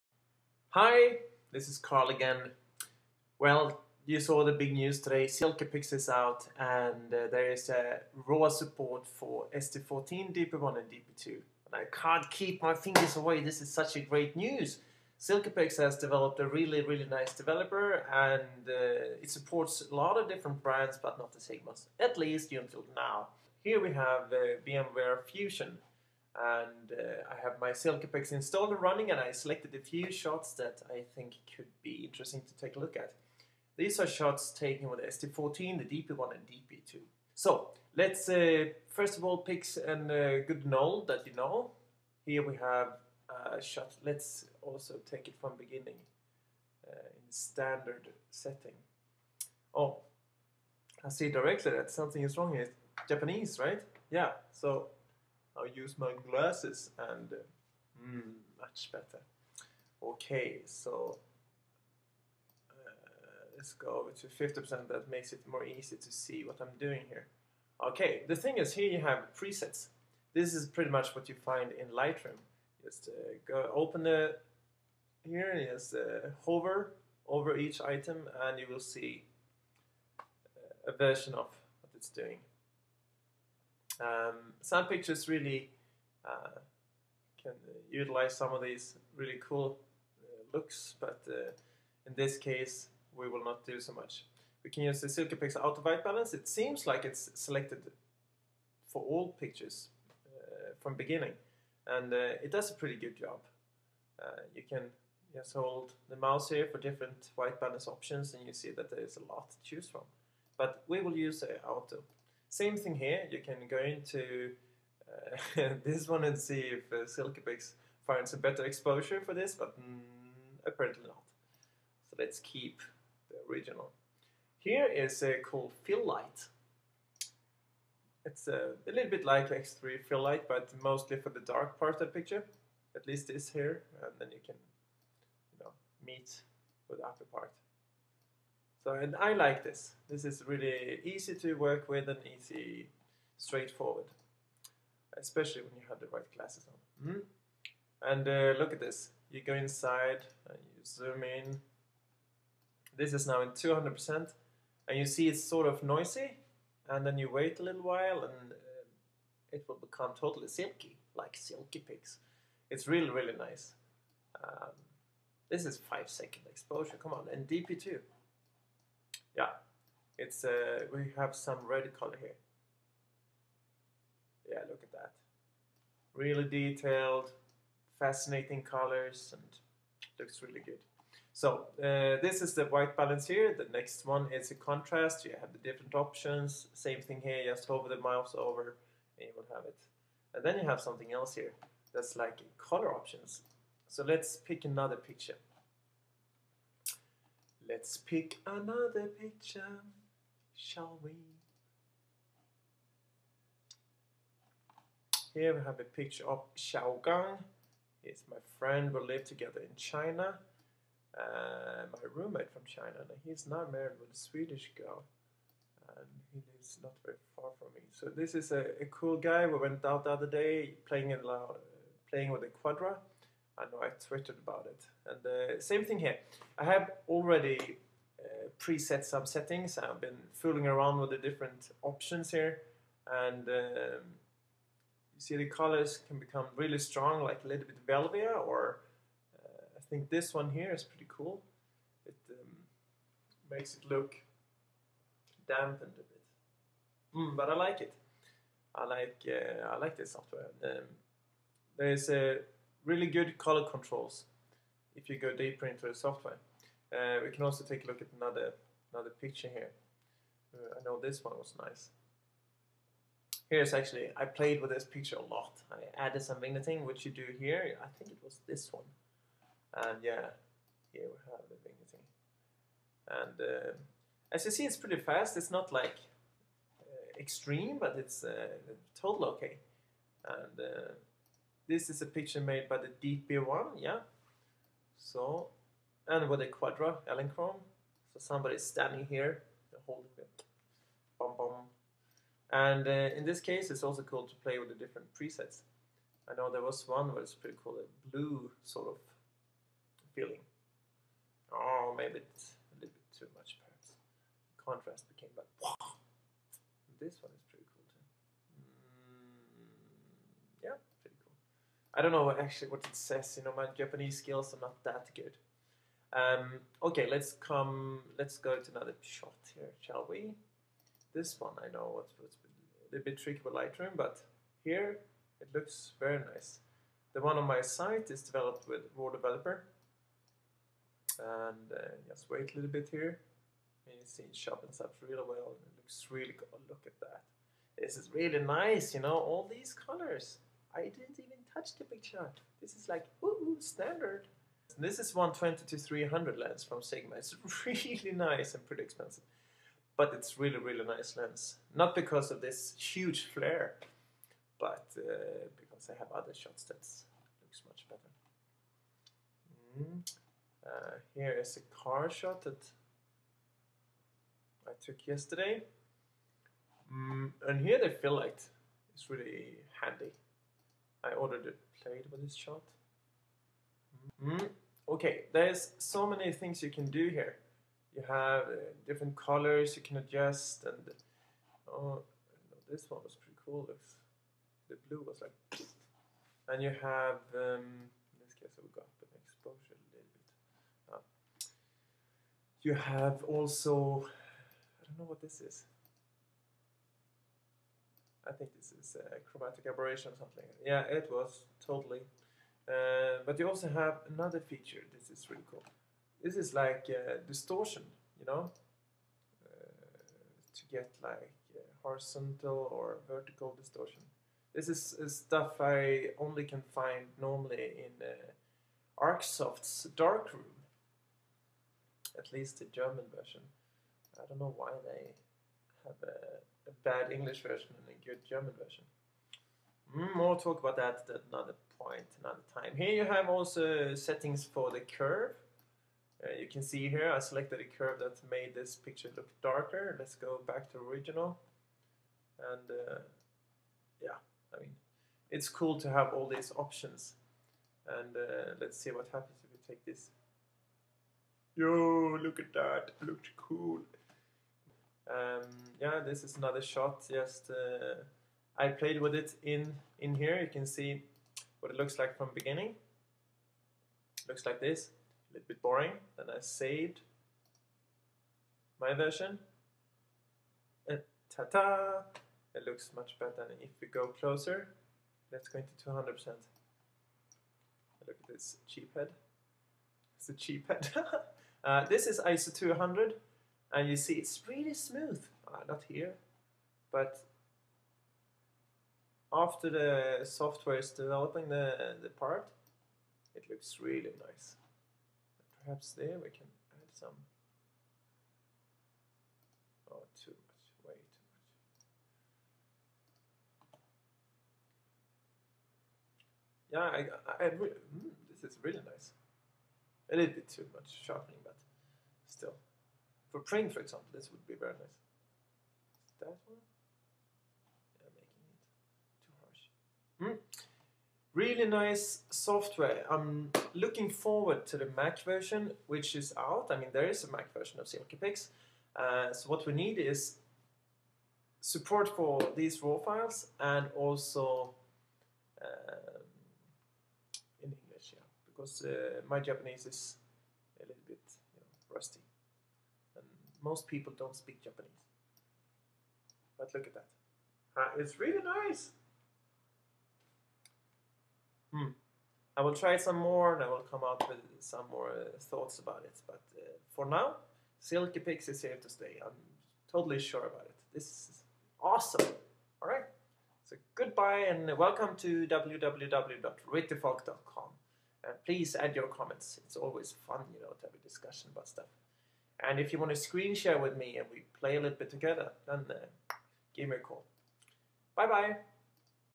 First look at Silkypix with Sigma X3F support! HD
SOUND FIXED! The wait is almost over. Silkypix has released a beta of their Silkypix developer pro software with Sigma SD14, DP1 and DP2 support. Here I run the Japanese version in my VMware fusion world. made